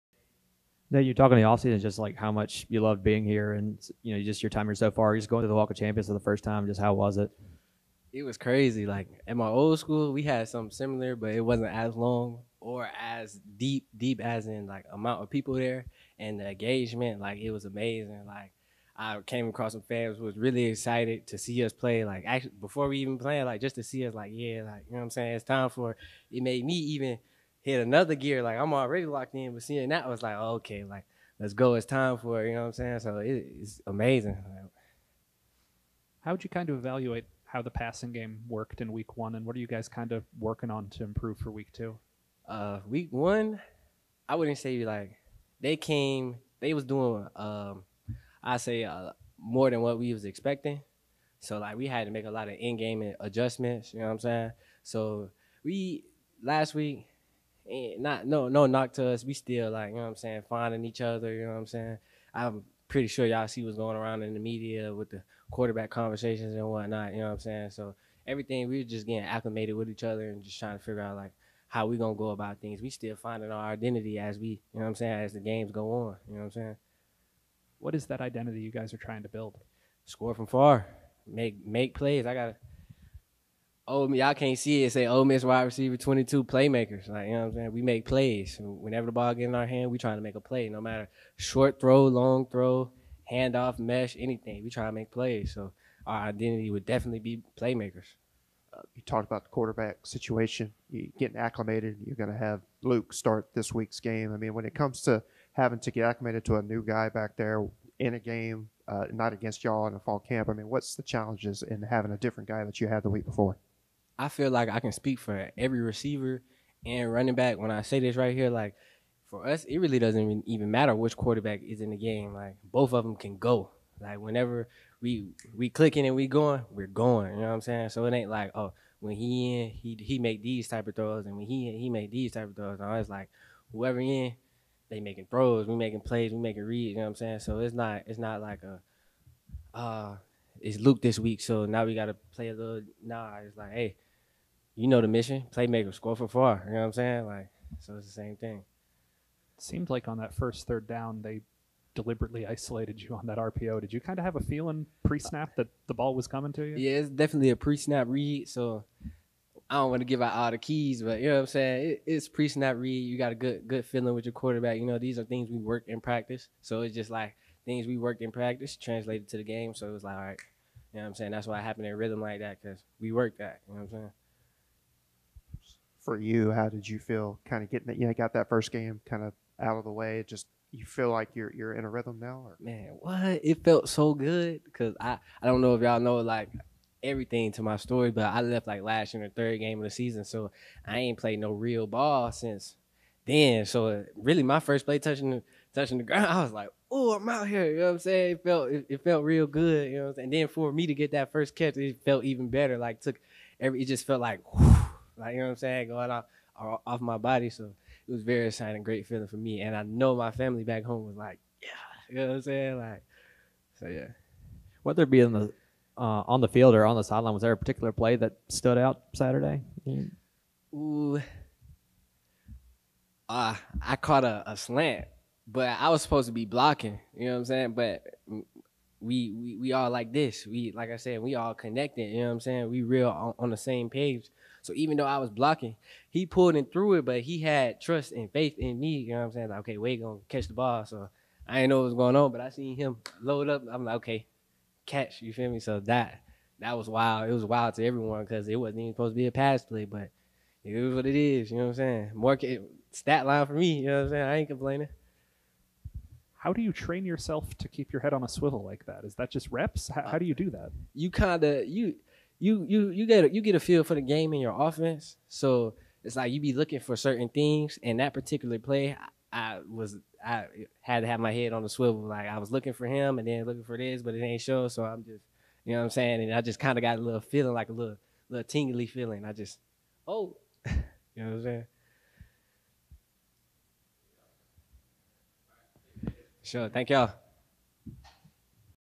Ole Miss WR Dayton Wade: September 6, 2022
Ole Miss wide receiver Dayton Wade talks with media after the Rebels' practice Tuesday following the 28-10 win over Troy. (Video: OMSP)